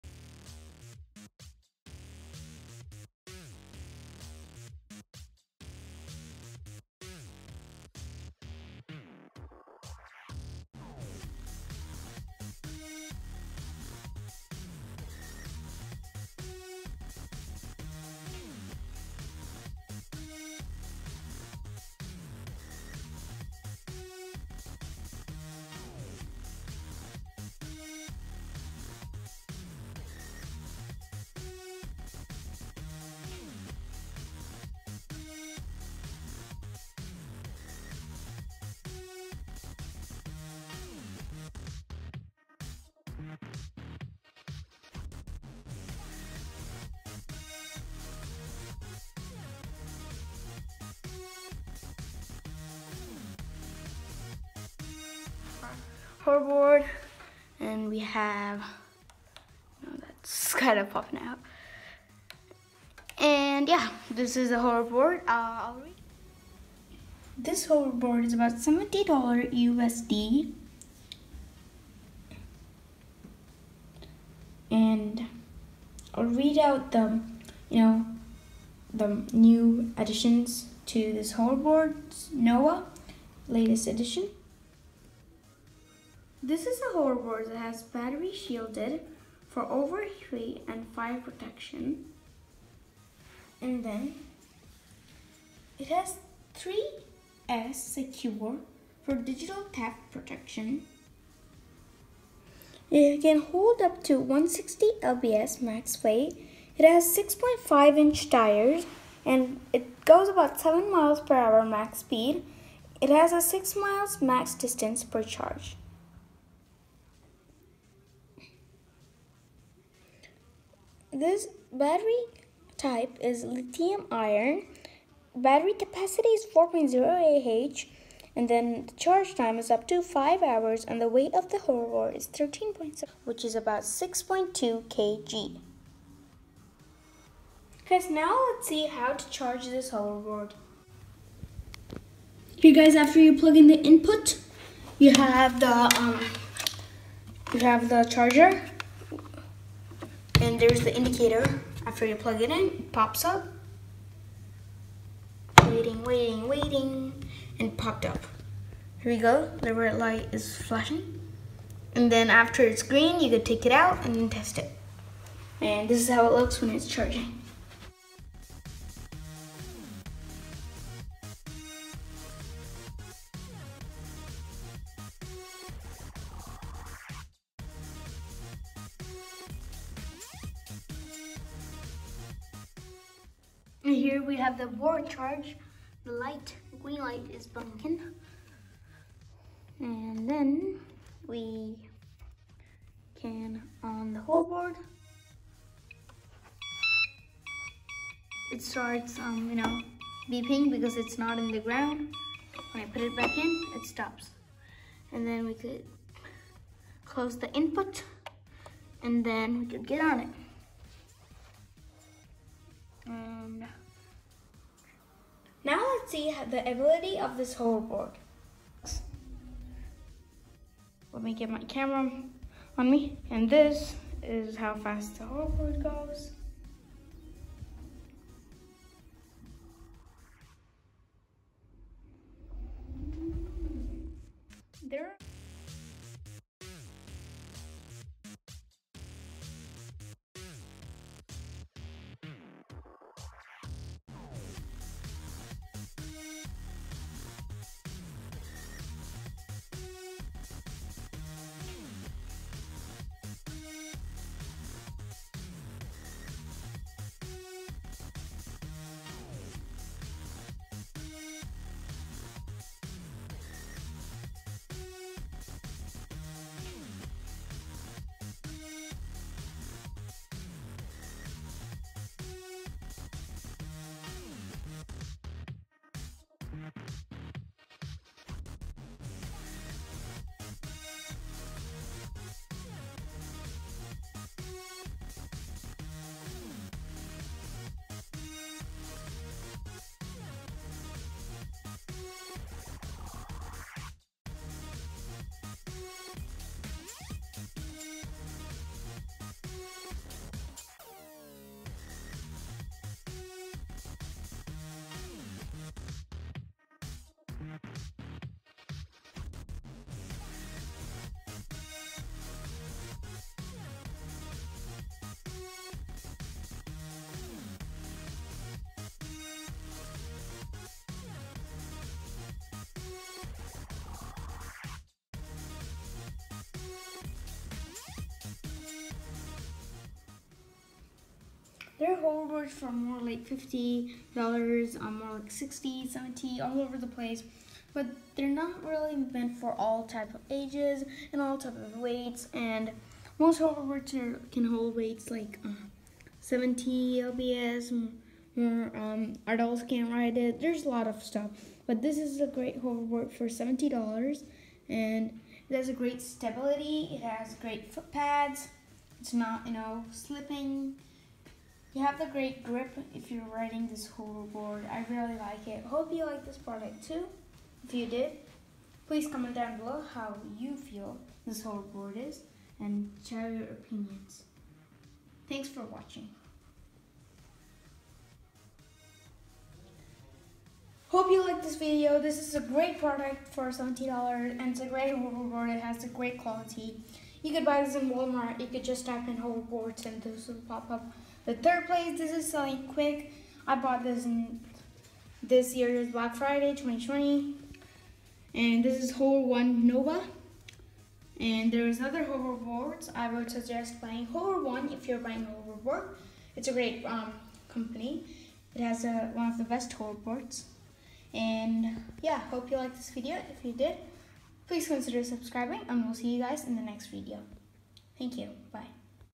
Hover1 nova review and testing| 7mph hoverboard| Info with prithvi.
This is a hoverboard for 70 dollar that goes about 7mph. Hope you like this video.